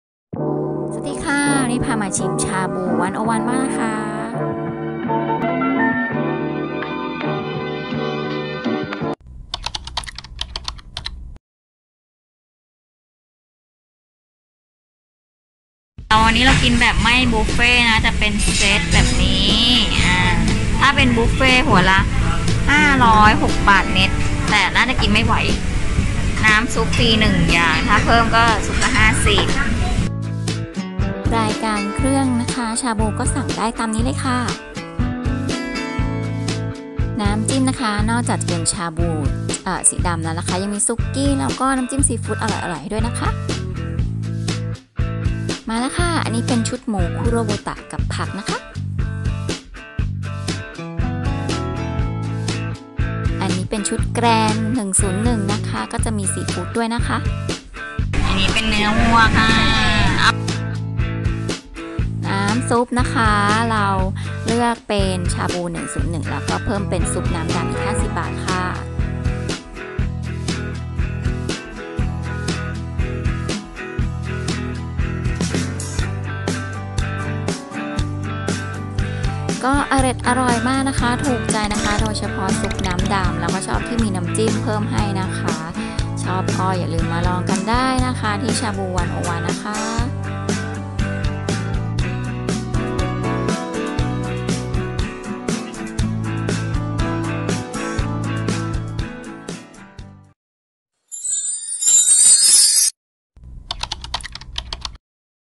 Shabu 101 ชาบู 101 อร่อยคุ้มๆ @ Central Silom Complex
Shabu 101
ชาบู 101
มาชิมชาบูร้านนี้กันบ้างค่ะ Shabu 101 น้ำซุปเลือกแบบครึ่งหม้อได้ จ่ายเพิ่ม 50 บาท มีแบบบุฟเฟ่ต์ และอะลาคาทนะคะ
เราไปกินแบบอะลาคาท เพราะคงกินบุฟเฟ่ต์ไม่คุ้มจ้า แค่นี้ก็อิ่มแล้ว จัดไป 2 ชุด ชอบตรงที่น้ำซุปดำ ไม่เค็มเกินไป ส่วนของก็สดใหม่คุ้มราคา ชุดผักในชุดชาบูถ้วยใหญ่ ถูกใจมากค่ะ น้ำจิ้มชาบู ปรุงเพิ่มเองตามชอบยังมีน้ำจิ้มสุกี้ และน้ำจิ้มซีฟู้ดรสชาติดีอีกด้วยค่ะ ชอบก็ไปลองกันได้ค่ะ

Shabu 101
ชาบู 101
ชั้น 4 สีลมคอมเพล็กซ์